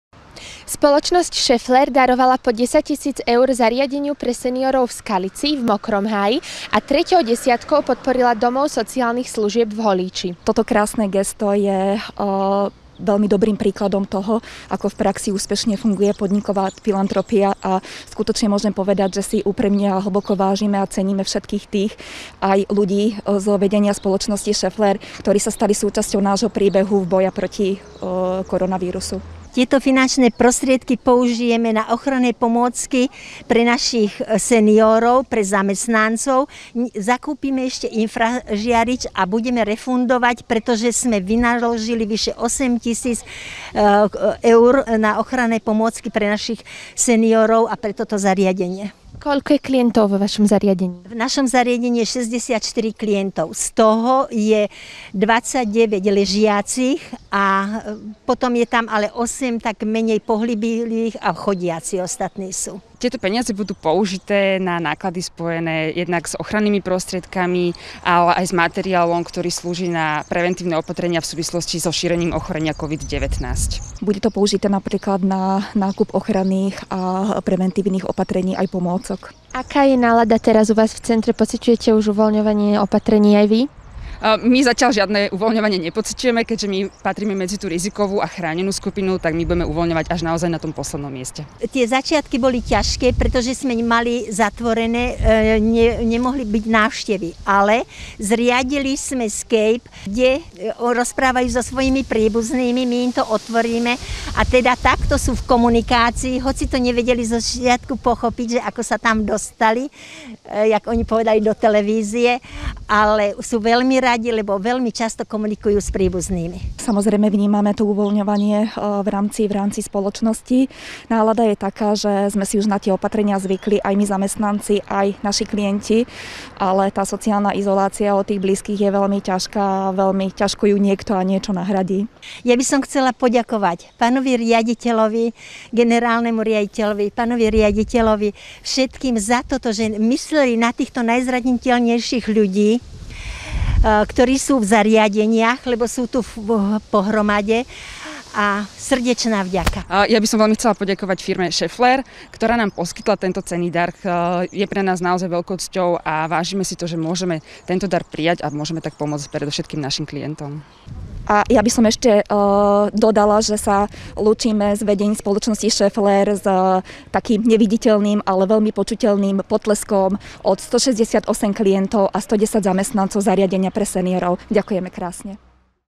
Schaeffler pomáha
Firma sa rozhodla venovať finančný dar pre zariadenia v Skalici, v Mokrom Háji a Holíči, aby počas pandémie pomohla práve najzraniteľnejšej skupine obyvateľstva